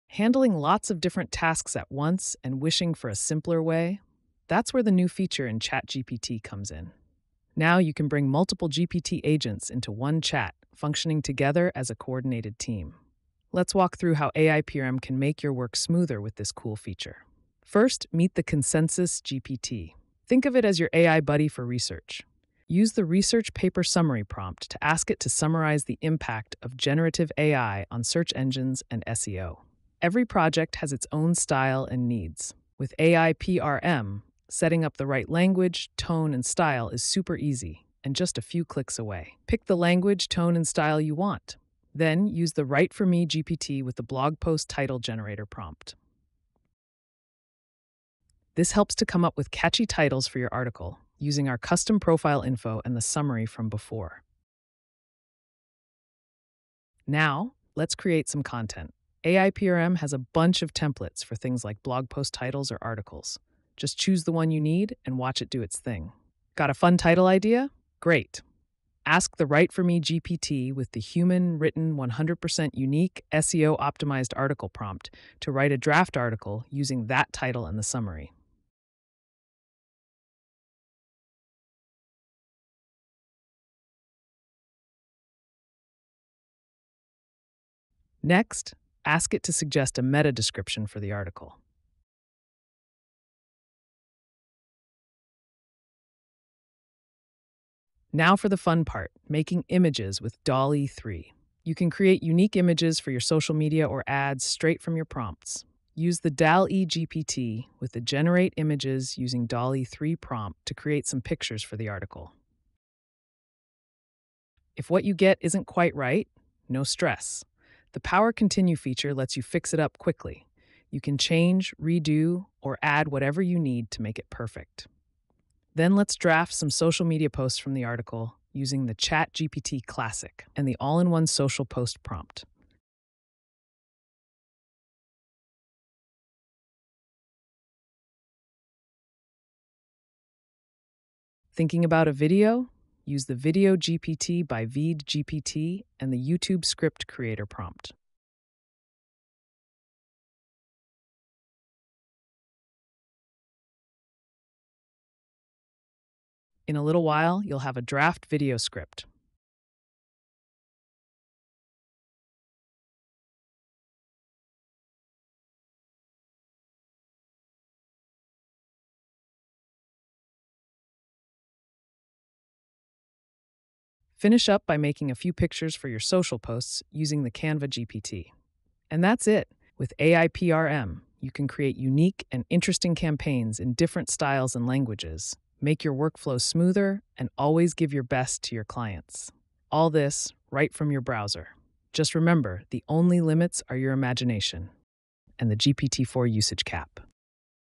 GPTs as Agents in ChatGPT with AIPRM - A quick Workflow Demo
Bring multiple GPT agents into one ChatGPT chat, functioning together as a coordinated team:

- "Consensus" GPT for smart, AI-powered research
- "Write For Me" GPT for content with adjustable language, tone, and style with AIPRM
- Bring your content to life with custom images from DALL-E 3
- Social media posts with "ChatGPT Classic"
- Video with "Video GPT by VEED"
- Visuals with "Canva" GPT

The only limits? Your imagination... and the GPT-4 usage cap 🫠